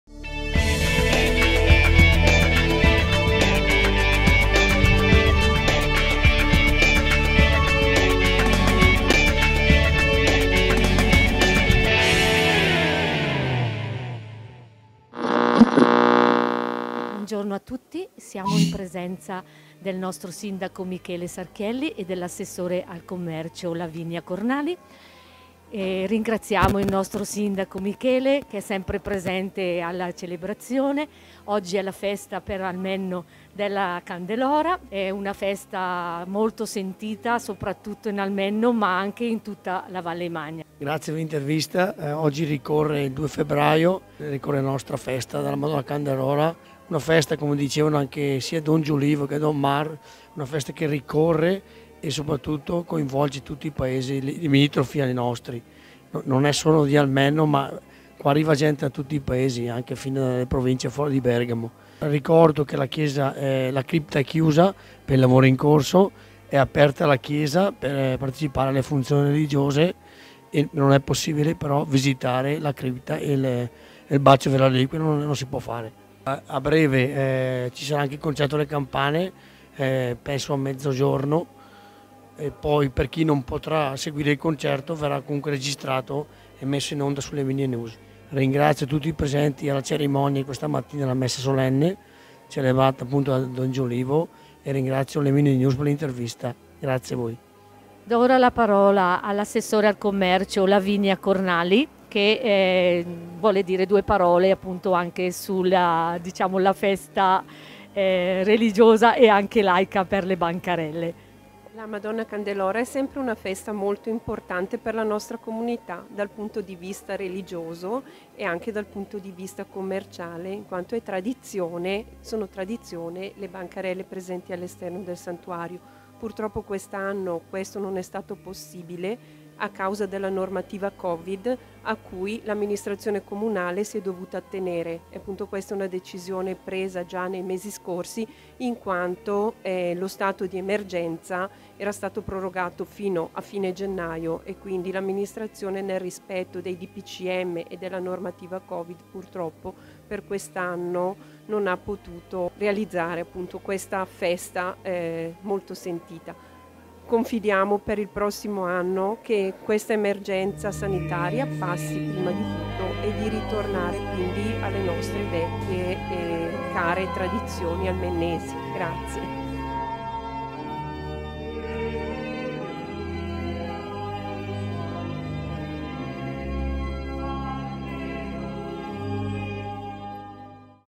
Intervista al Sindaco Michele Sarchielli e All'Assessore Lavinia Cornali - Madonna Candelora 2021
La nostra inviata Elvira Zanchi in occasione delle Celebrazioni della Madonna Candelora intervista il Sindaco di Almenno San Salvatore Michele Sarchielli e l’Assessore  al Bilancio – Tributi - Commercio e Attività produttive Lavinia Cornali.
Per iscrivervi al gruppo  di LemineNews cliccate il link che trovate quì sotto: